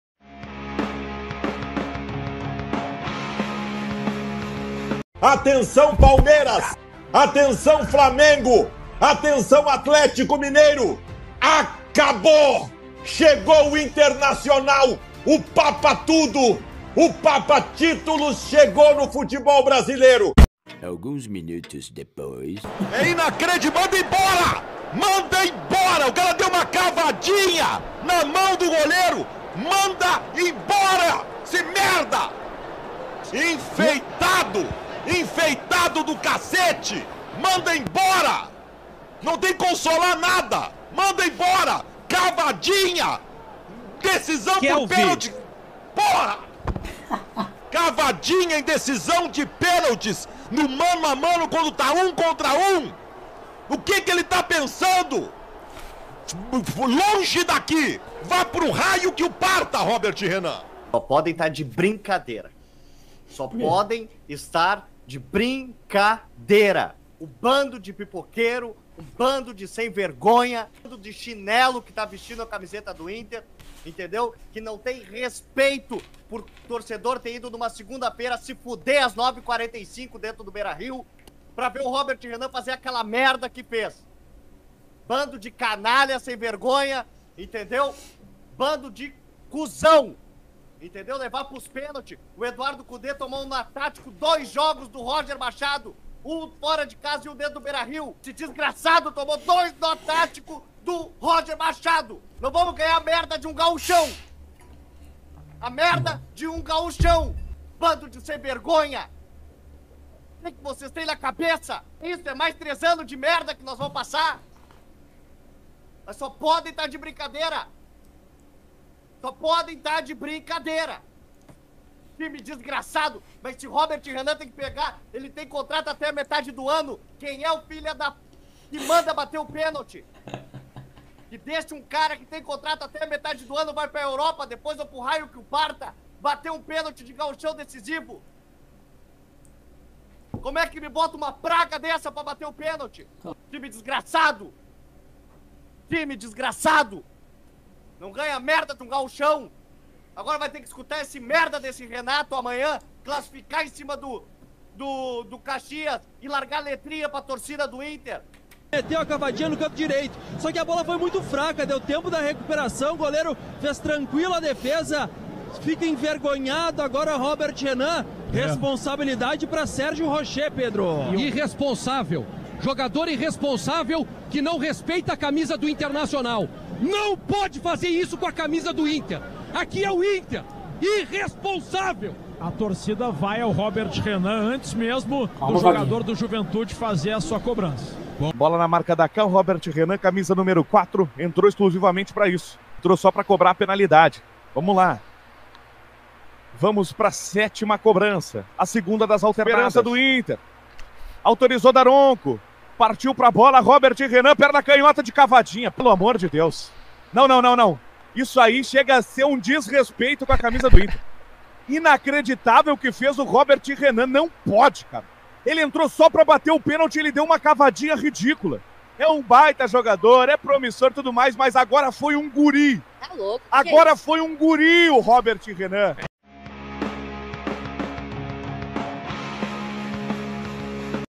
INTER É ELIMINADO PELO JUVENTUDE E COLORADOS VÃO A LOUCURA.
LINKS